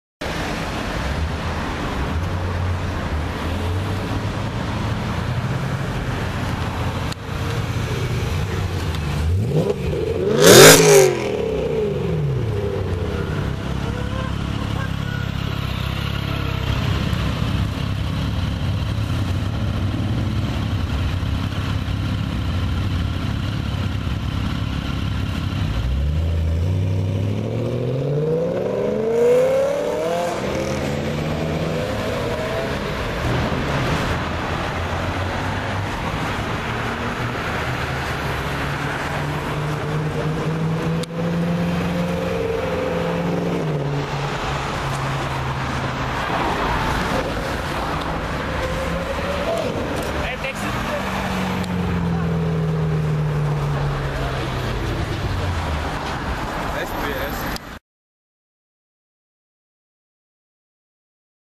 BMW M3 Convertible revs & sounds HD
this is a nice BMW M3 Convertible. I hope you like this video. Most of my Videos made at the Motorworld near Stuttgart (Germany).